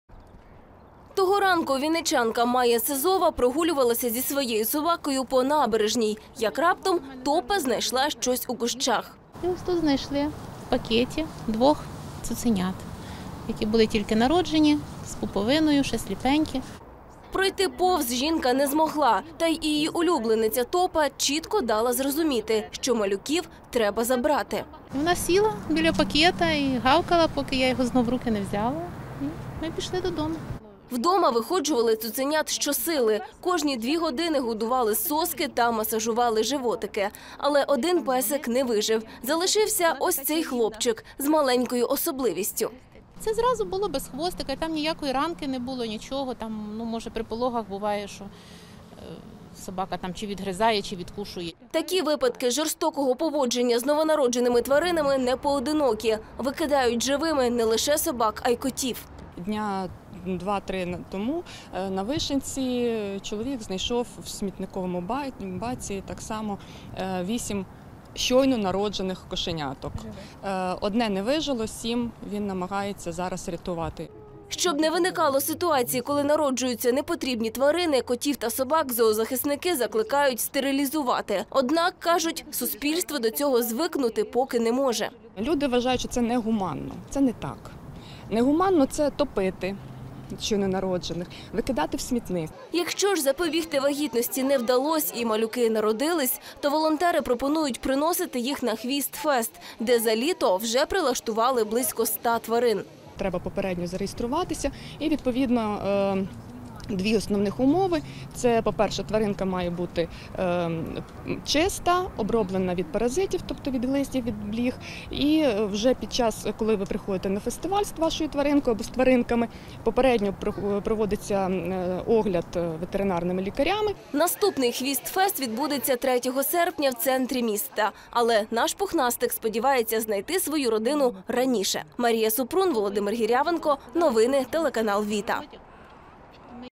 Безхвоста знахідка, новини 2019-07-26
Народився без хвостика, але в сорочці. Цуцика, якого прирекли на вірну смерть, знайшла та виходила вінничанка. Нещодавно народженого цуценяту викинули, як сміття та ще й зав’язали в поліетиленовий пакет. Сьогодні крихітний пухнастик зміцнився, підріс та шукає нову родину.